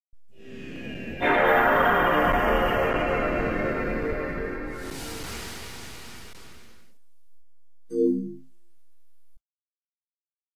PlayStation 2 Startup Boot (HD 1080)
Boot Loader Playstation 2 on PCSX2 Emulator.